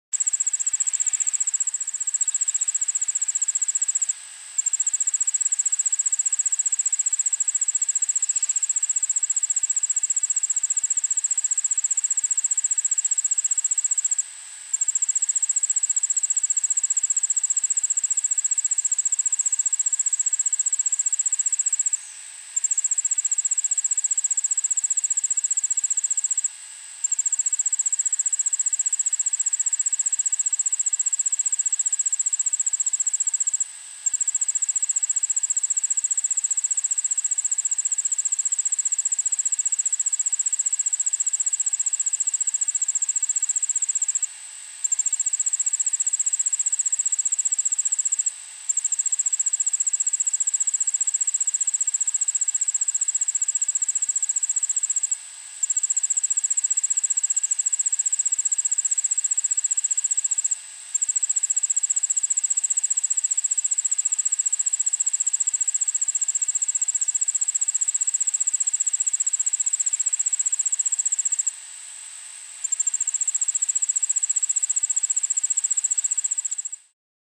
Allard's Ground Cricket (Allonemobius allardi)
Allard's Ground Cricket (Allonemobius allardi) singing.
Montoursville, PA. August 9th, 2016, 1pm
Suburban backyard

Recorded with Sony PCM-M10 and Sennheiser shotgun microphone